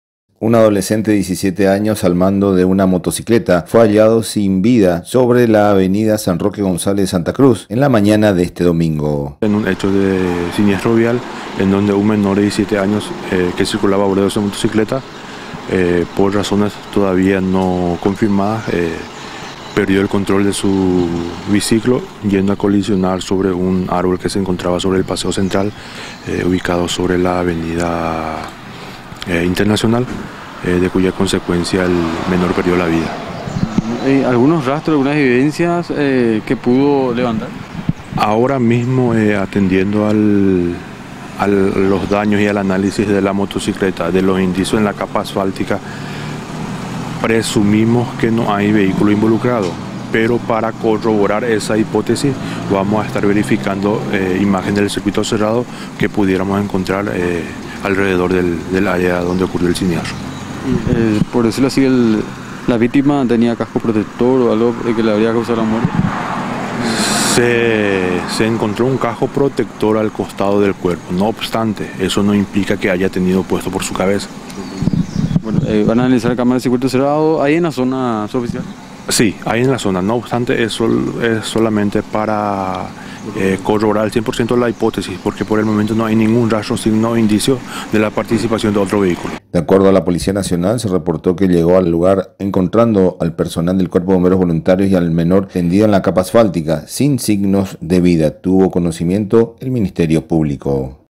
Hallan sin vida a motociclista sobre la Avda. San Roque de Encarnación
Un adolescete de 17 años al mando de su motocicleta fue hallado sin vida sobre la avenida San Roque González de Encarnación en la mañana de este domingo.